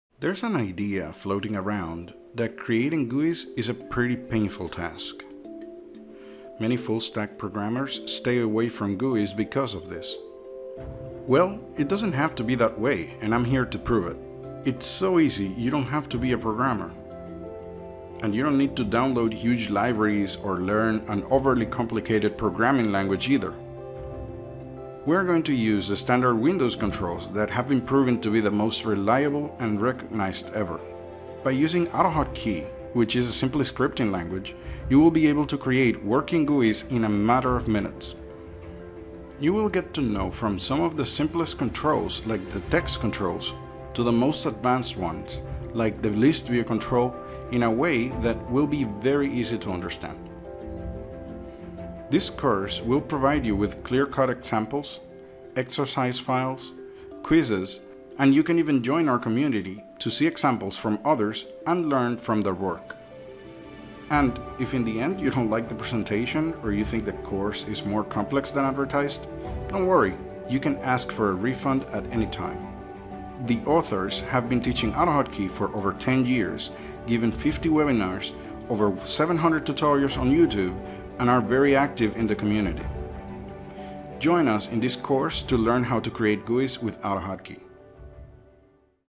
New Udemy course on how to create GUIs with Autohotkey
This is a beginners level course that I was developing with Joe Glines.

The course is very easy to follow, and starts with the assumption that you know absolutely nothing about programming GUIs.

We will cover from the most basic controls to the most advanced ones in a very clear way. And you'll soon notice that programming GUIs in AHK is way easier that in other languages, even kivy!